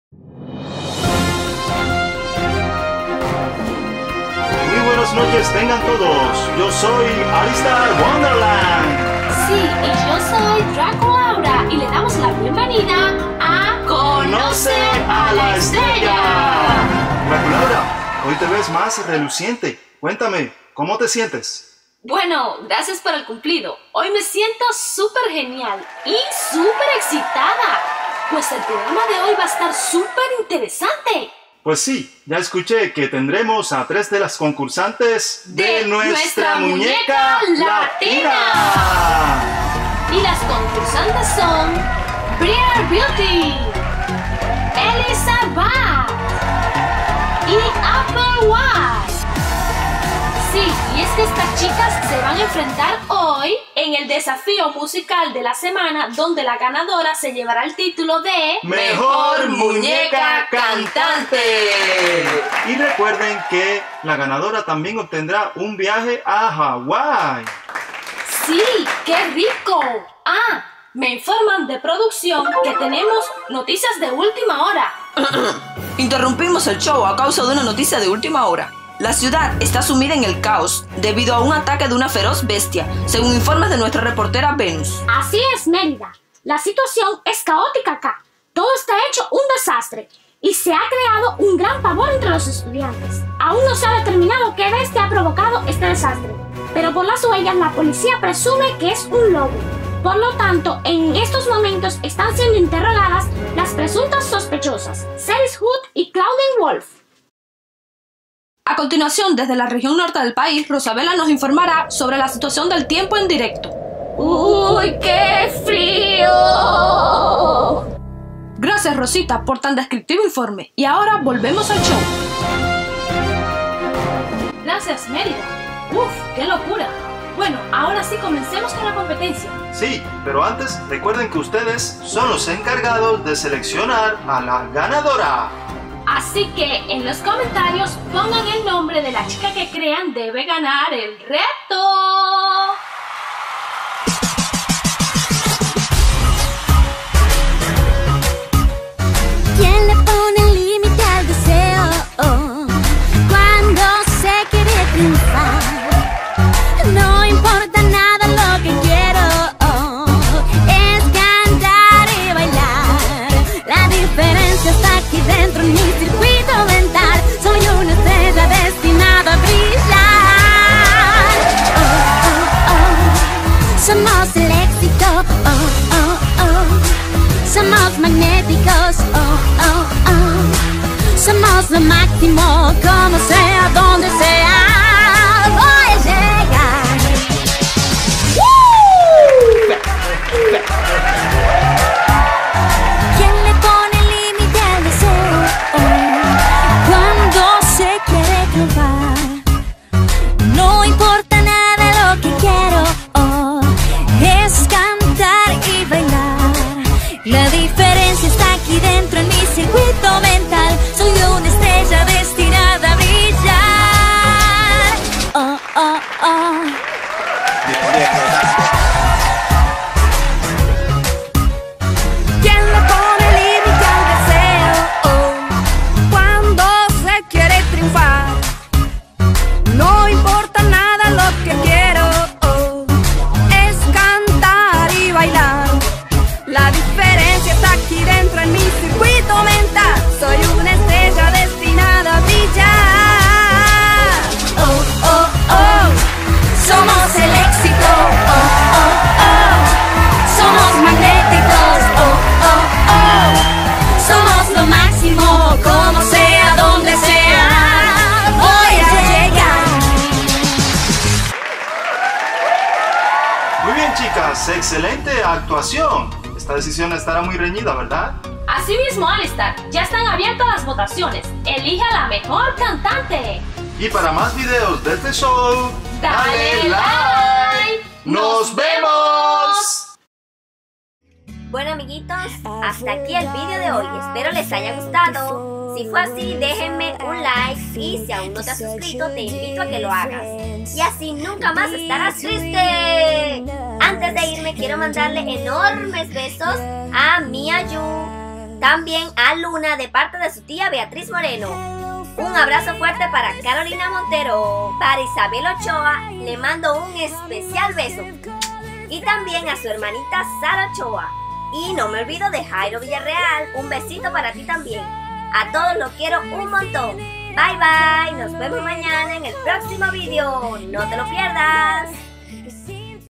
competencia de canto | jugues de ever after high y muñecas monster high
Las muñecas de ver after high Apple White, Briar Beauty  y la muñeca de monster high Elissabat hoy se enfrentan a una competencia de canto donde la ganadora recibirá un gran premio en la novela de muñecas siguiente. Este programa es conducido por los juguetes de ever after high y monster high Alistair y Draculaura respectivamente.